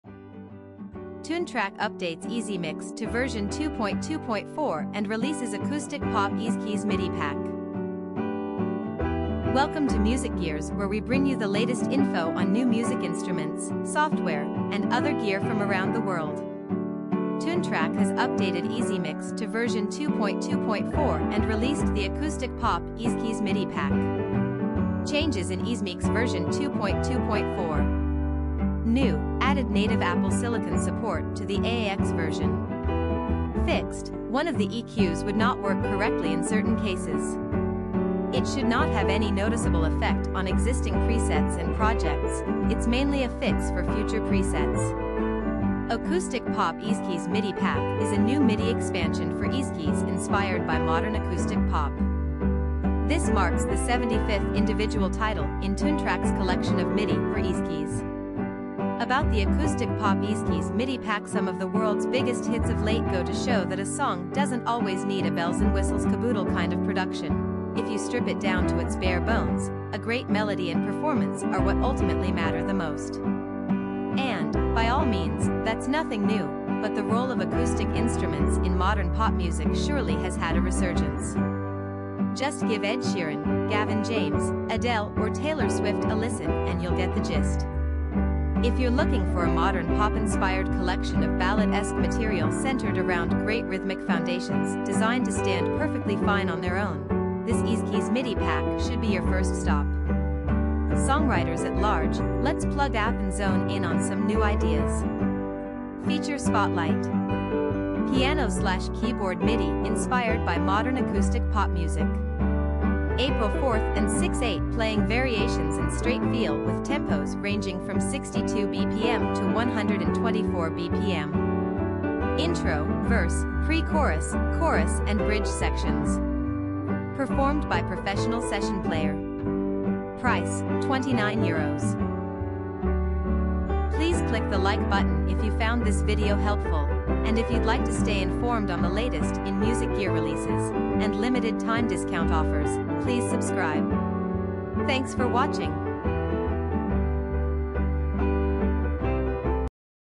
Toontrack updates EZMix to v2.2.4 and releases Acoustic Pop EZkeys MIDI pack
Toontrack has updated EZMix to v2.2.4 and released the Acoustic Pop EZkeys MIDI pack.

Changes in EZmix v2.2.4:

New: Added native Apple silicon support to the AAX version.
Fixed: One of the EQs would not work correctly in certain cases. It should not have any noticeable effect on existing presets and projects, it's mainly a fix for future presets.
Acoustic Pop EZkeys MIDI pack is a new MIDI expansion for EZkeys inspired by modern acoustic pop. This marks the 75th individual title in Toontrack's collection of MIDI for EZkeys.

About the Acoustic Pop EZkeys MIDI packSome of the world's biggest hits of late go to show that a song doesn't always need a bells-and-whistles-caboodle kind of production – if you strip it down to its bare bones, a great melody and performance are what ultimately matter the most. And, by all means, that's nothing new – but the role of acoustic instruments in modern pop music surely has had a resurgence. Just give Ed Sheeran, Gavin James, Adele or Taylor Swift a listen and you'll get the gist.

If you're looking for a modern pop-inspired collection of ballad-esque material centered around great rhythmic foundations designed to stand perfectly fine on their own, this EZkeys MIDI pack should be your first stop. Songwriters at large, let's plug out and zone in on some new ideas.

Feature Spotlight:

Piano/keyboard MIDI inspired by modern acoustic pop music.
4/4 and 6/8 playing variations in straight feel with tempos ranging from 62 BPM to 124 BPM.
Intro, verse, pre chorus, chorus and bridge sections.
Performed by professional session player.
Price: €29